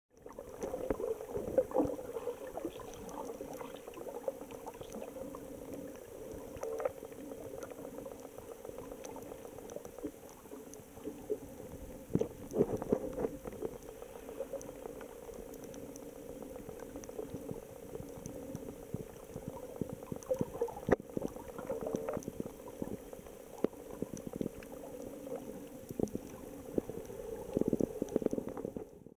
透明な海でお魚追跡☆2013竹野浜
白い砂浜と透明な海が美しい兵庫県 竹野浜。
陽の光がさし込む海の中をゆらゆらただよっているだけで癒されます。
沖のほうにあるテトラポット周辺は貝や魚の住処となっていて、
また違った楽しみ方が出来ます。

ブログでも詳しくレポートしています。